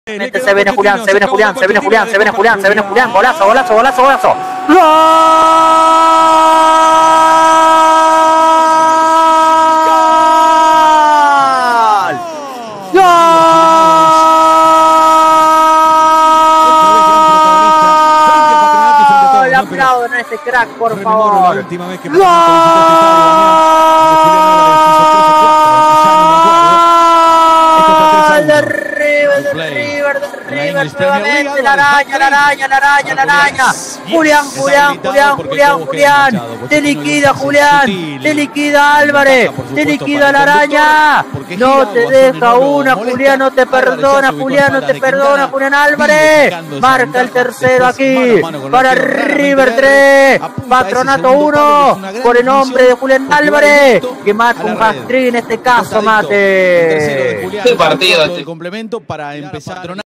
RIVER 4 - 1 PATRONATO (Gol de Julián Álvarez 3ro)
🎤⚽️Mi relato del tercer gol de Julián Álvarez en el Monumental 🏟 por la Fecha N° 2 Zona A de la CopaDeLaLiga 2022 🏆.

🎙Lo viviste por @falsonueve_f9.